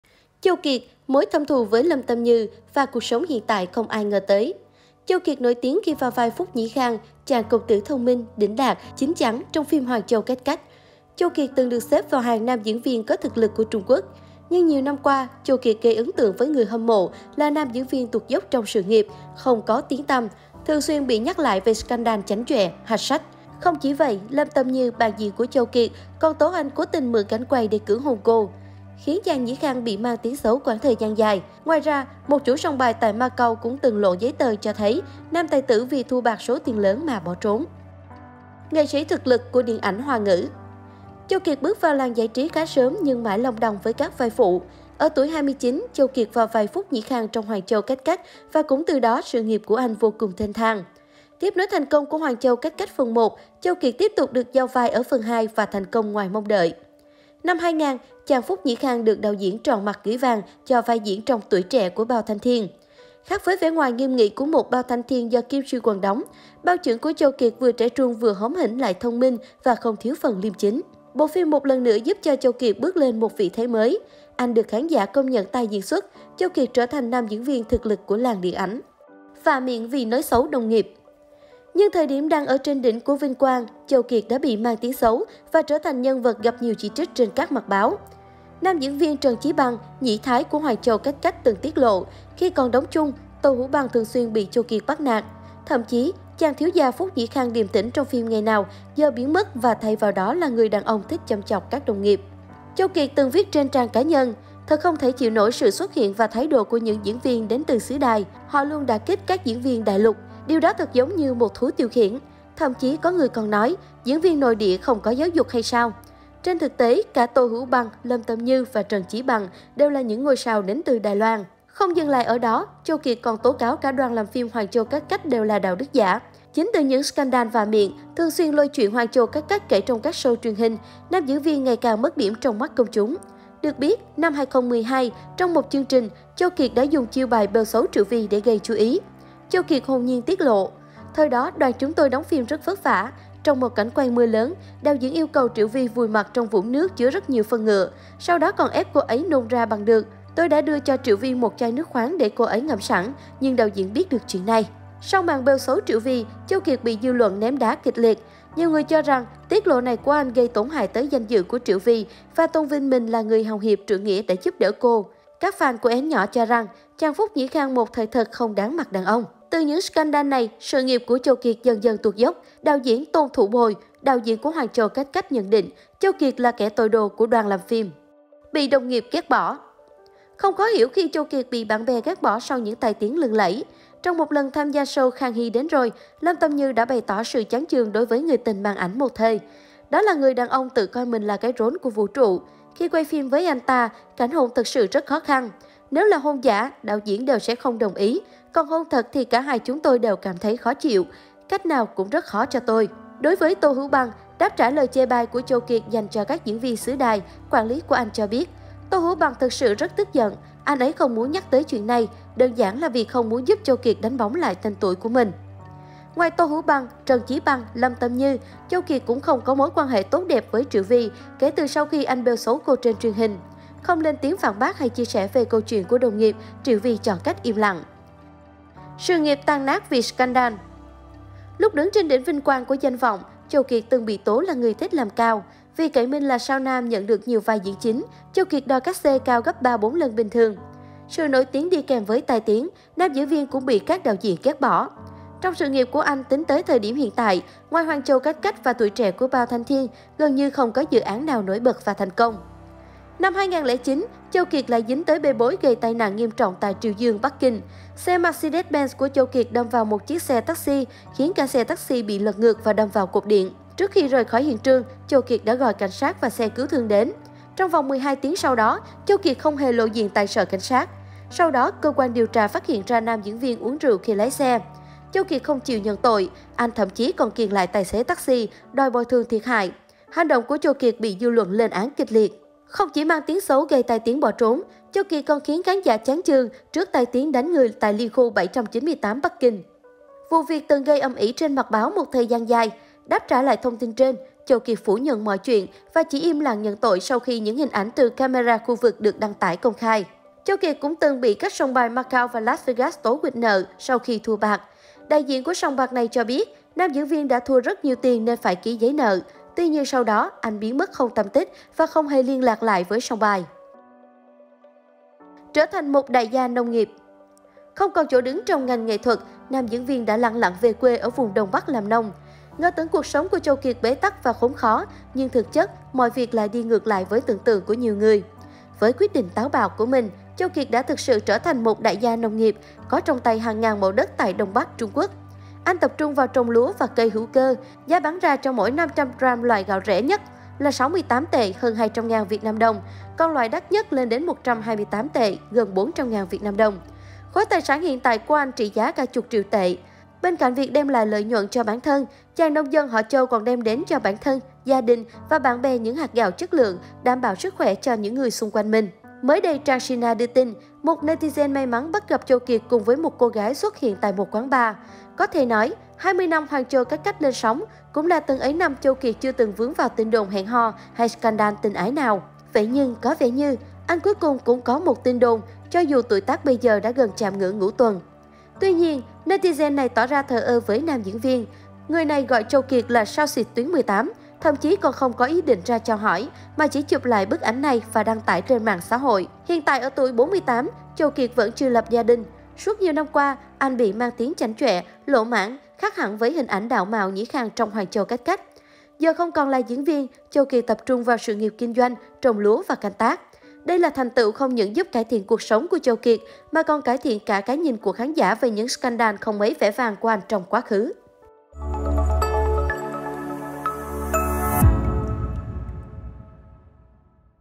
Châu Kiệt - Mối thâm thù với Lâm Tâm Như và cuộc sống hiện tại không ai ngờ tới
Châu Kiệt nổi tiếng khi vào vai Phúc Nhĩ Khang, chàng công tử thông minh, đĩnh đạc, chín chắn trong phim Hoàn Châu cách cách. Châu Kiệt từng được xếp vào hàng nam diễn viên có thực lực của Trung Quốc. Nhưng nhiều năm qua  Châu Kiệt gây ấn tượng với người hâm mộ là nam diễn viên tuột dốc trong sự nghiệp, không còn tiếng tăm, thường xuyên bị nhắc lại về scandal chảnh chọe, hạch sách. Không chỉ vậy, Lâm Tâm Như - bạn diễn của Châu Kiệt - còn tố anh cố tình mượn cảnh quay để cưỡng hôn cô, khiến chàng Nhĩ Khang bị mang tiếng xấu quãng thời gian dài. Ngoài ra, 1 chủ sòng bài tại Macau cũng từng lộ giấy tờ cho thấy nam tài tử vì thua bạc số tiền lớn mà bỏ trốn.